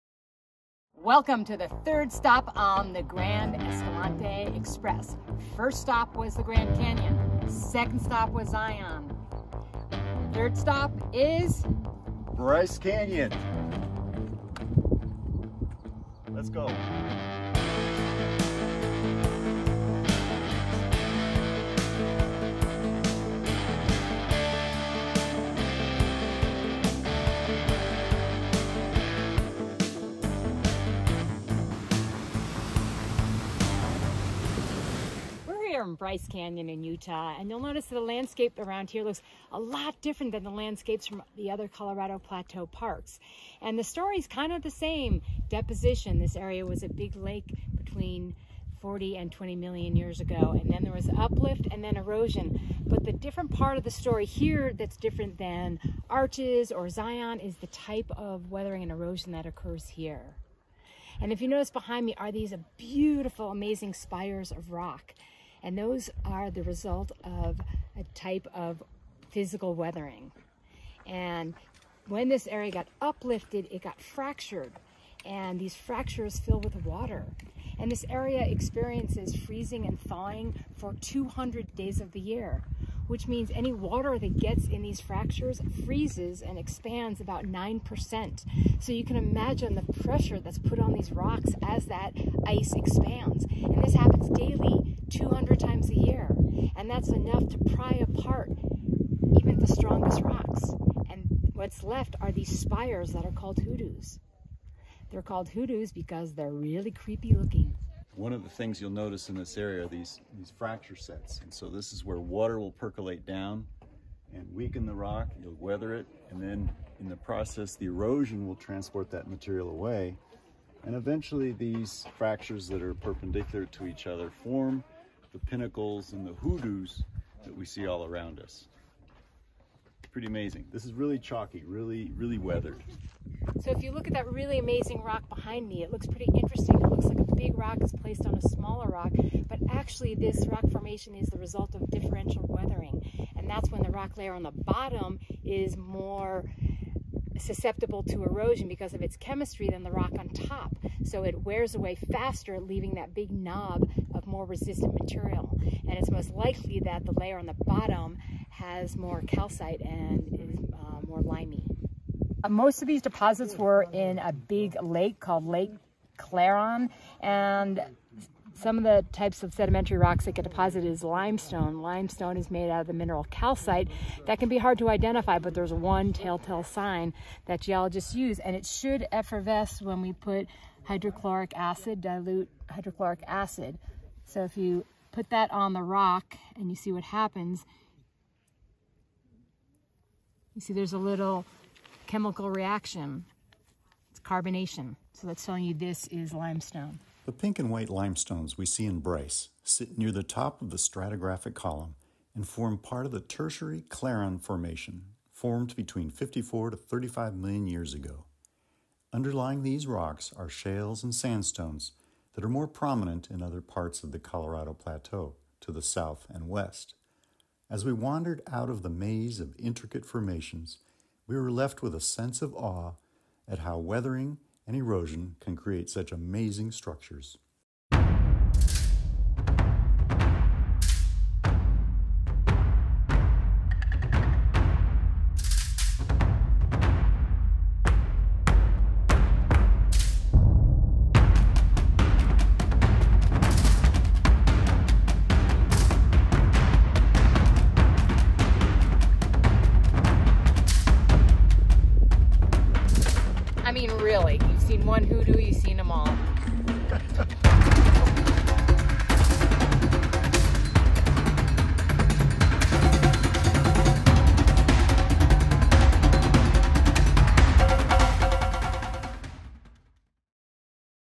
Bryce Canyon National Park - The Land of Hoodoos
Welcome to Bryce Canyon National Park, the stunning "Land of the Hoodoos!" Join us, a team of passionate geologists, as we explore the unique landscapes, vibrant colors, and fascinating geology of this iconic park. Learn how these towering rock formations, known as hoodoos, were shaped by millions of years of weathering and erosion. Discover the science behind Bryce Canyon and the stories told by its rocks. Whether you're planning a visit or just curious about Earth's natural wonders, this video is for you!

Let us know your favorite part of Bryce Canyon in the comments!

Thanks,
-myScienceBlast